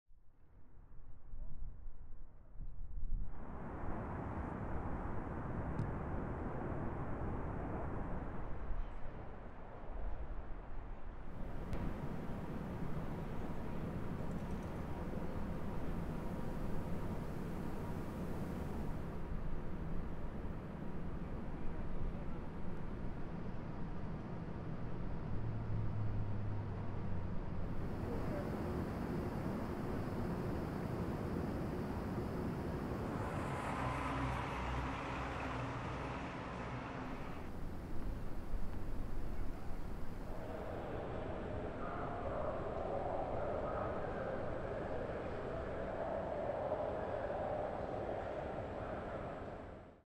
شاهد: نيويورك، صمت في شوارع مدينة لم تتعود على النوم
قال الرئيس دونالد ترامب إن ألف جندي، معظمهم من الأطباء والممرضات، سيتم نشرهم في مدينة نيويورك لمساعدة السكان على تجاوز أزمة كورونا.